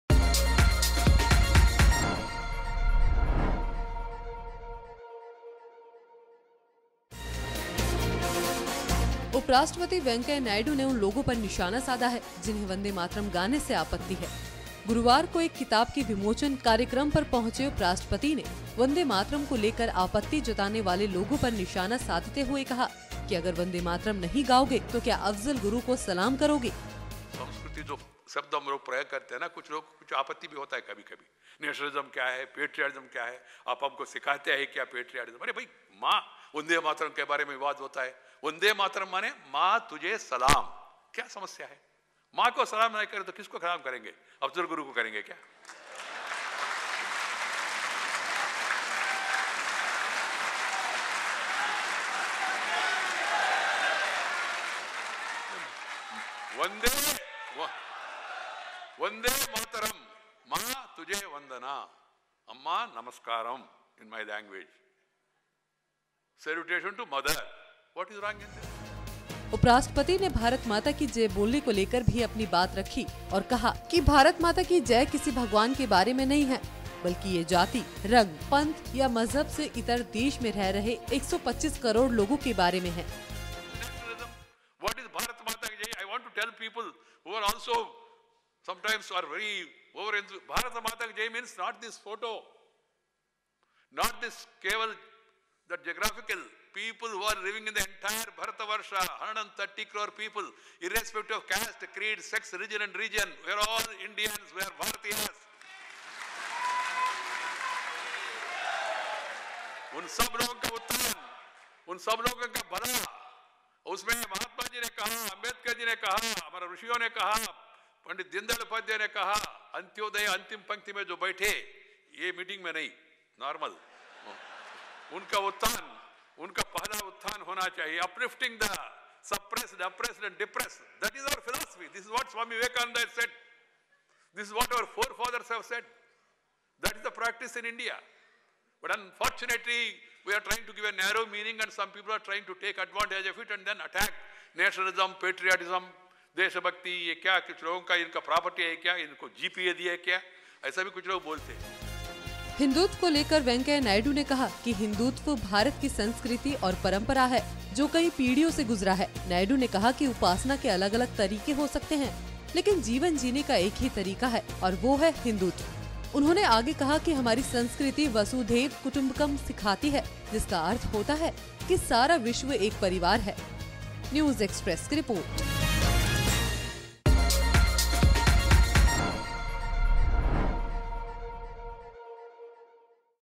मां नहीं तो क्या अफजल गुरु को सलाम करोगे?- Venkaiah Naidu
"If not your mother, who would you salute? Afzal Guru?" Venkaiah Naidu said at an event to release a book on the late VHP chief Ashok Singhal.

producer - prakash
video editor - Vineet singh